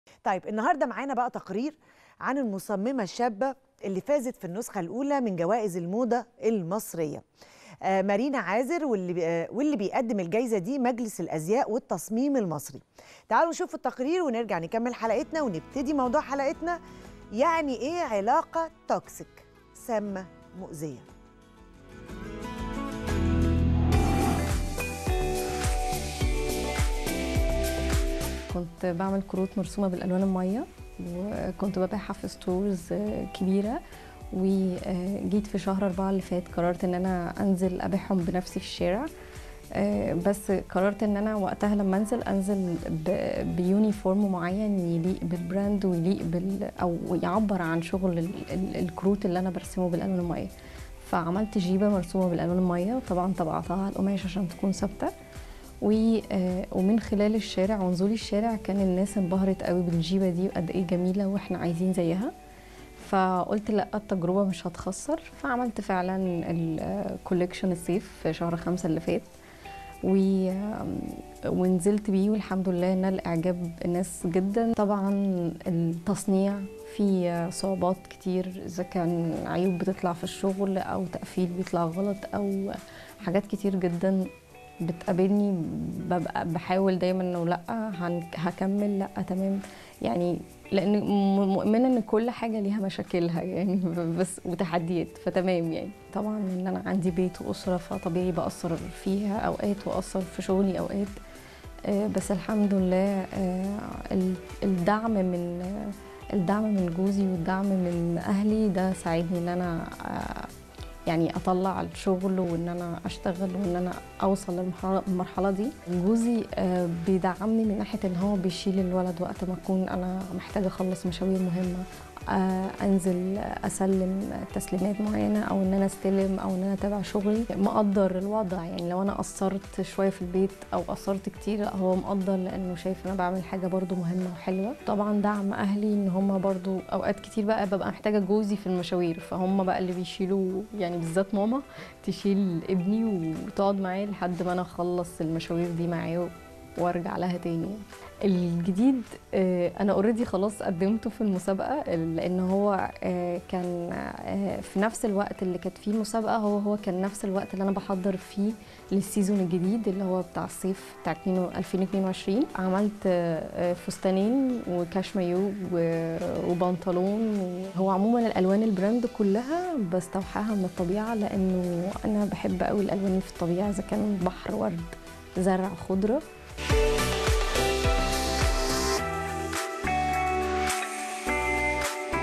من الرسم بالألوان المائية إلى جائزة مجلس الأزياء والتصميم المصري ..قصة تحدي ونجاح مارينا عازر
يحكى أن | من الرسم بالألوان المائية على الكروت إلى جائزة مجلس الأزياء والتصميم المصري ..قصة تحدي ونجاح من مارينا عازر